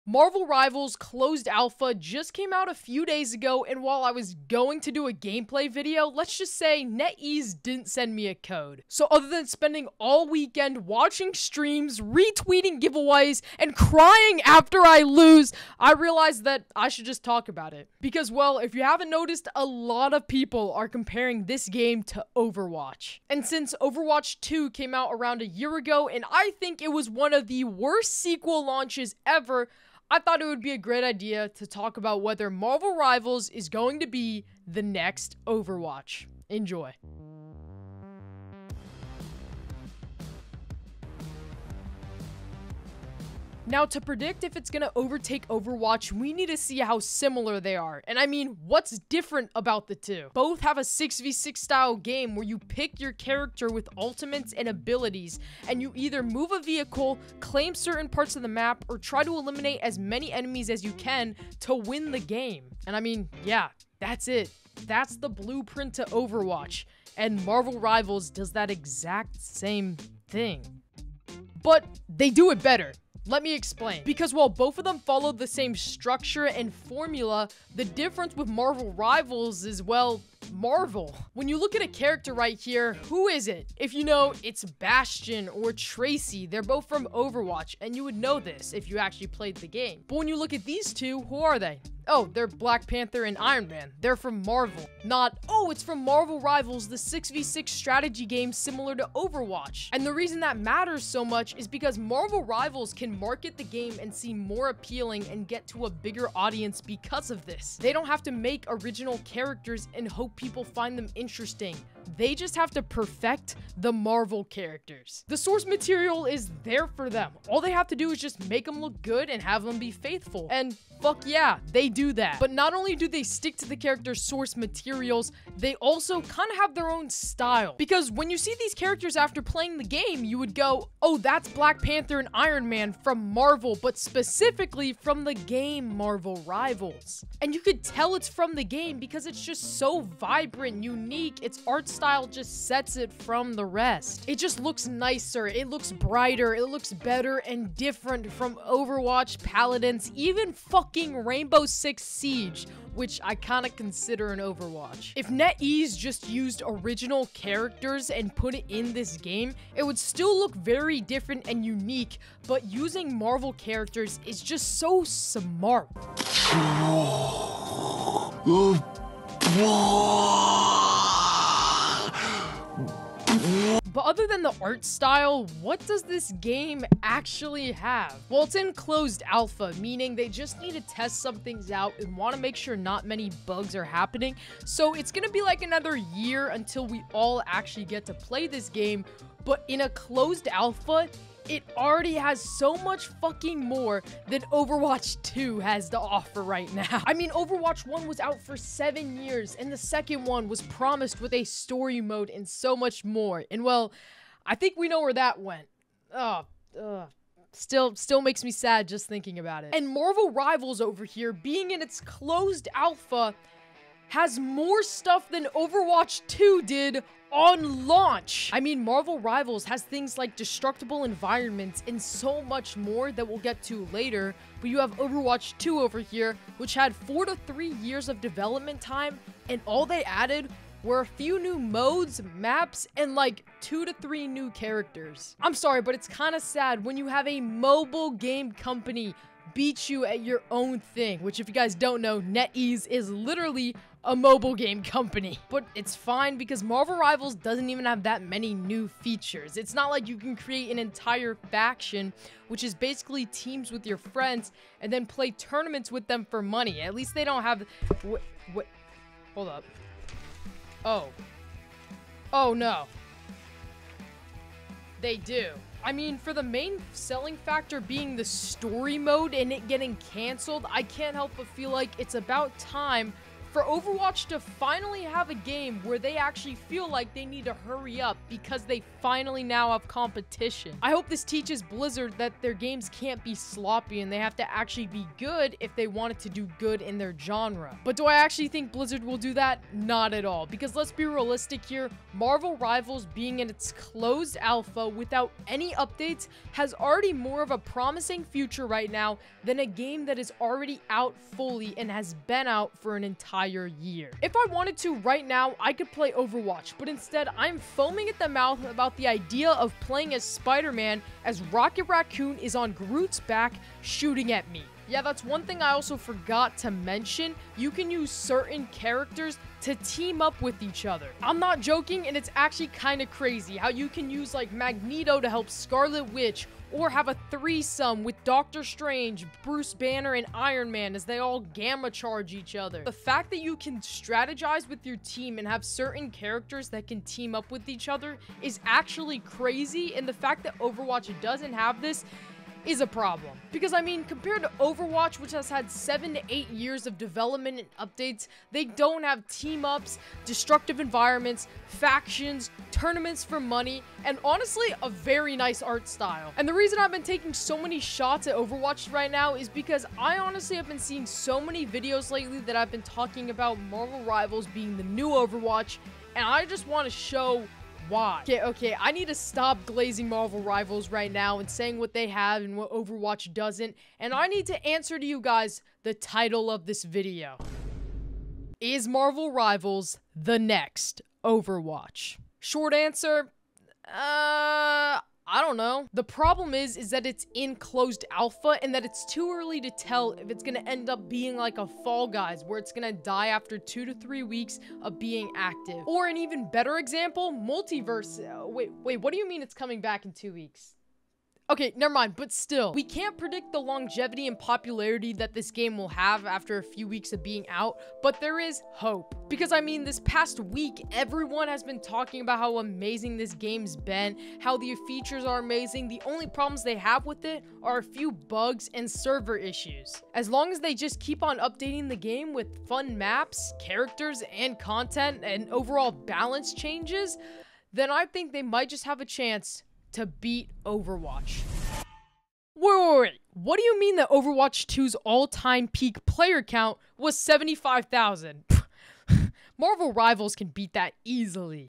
Is Marvel Rivals The Next Overwatch??
Marvel rivals closes alpha just came out and since i didnt get a code i decided im going to talk about it. And wether or not its going to be the next overwatch?